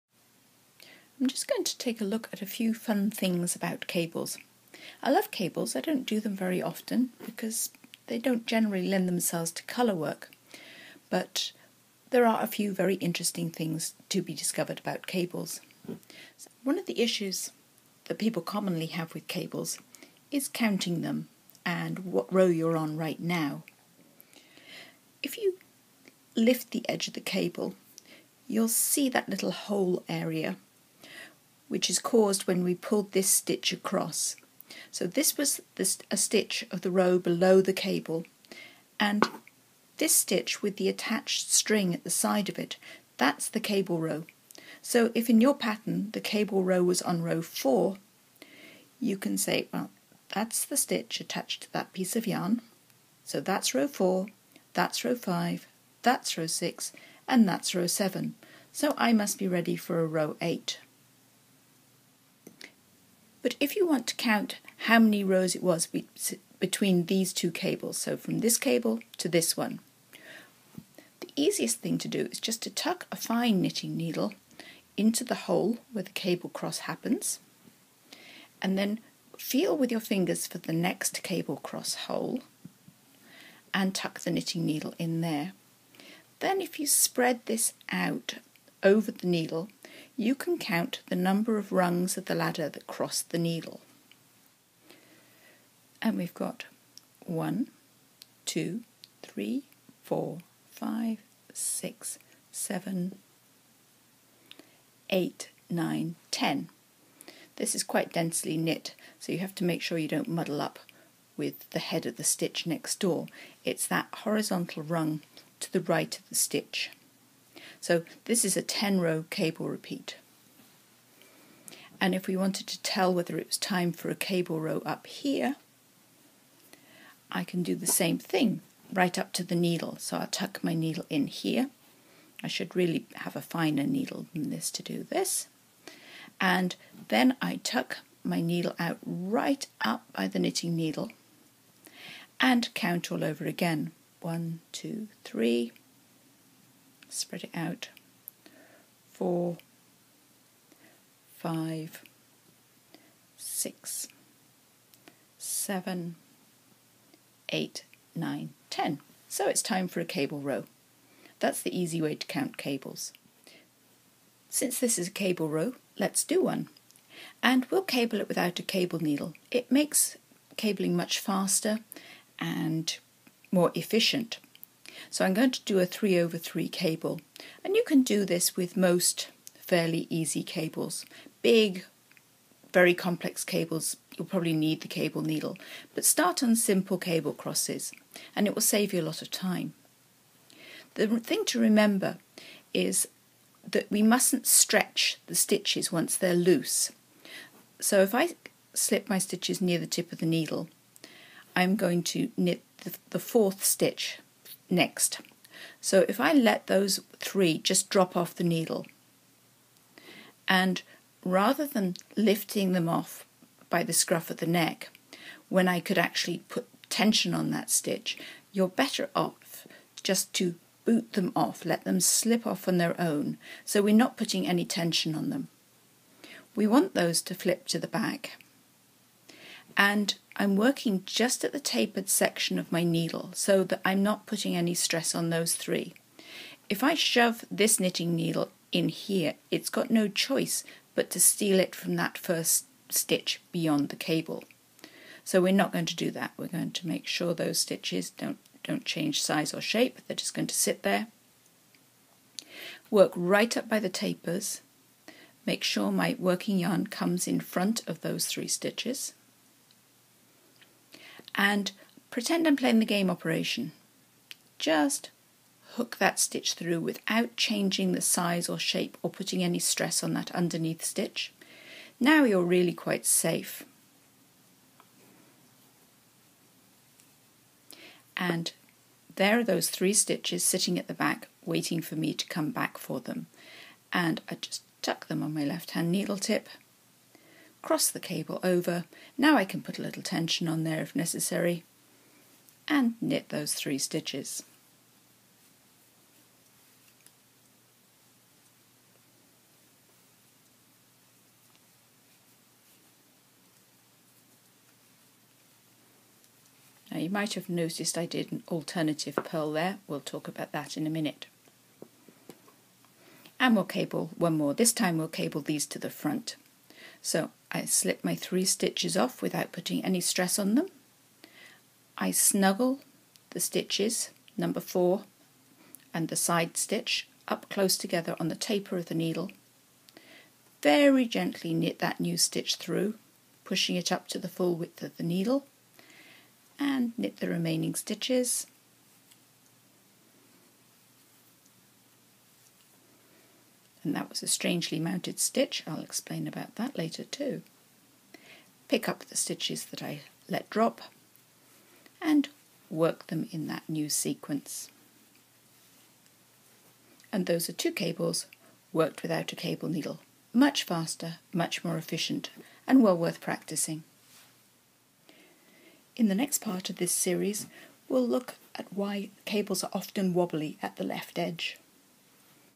Cable Fun - Part 1
How to easily count cable rows and repeats and cable without an extra needle with Lucy Neatby.

For more Tutorials, Patterns, Books and my "Learn with Lucy" DVD series, please visit my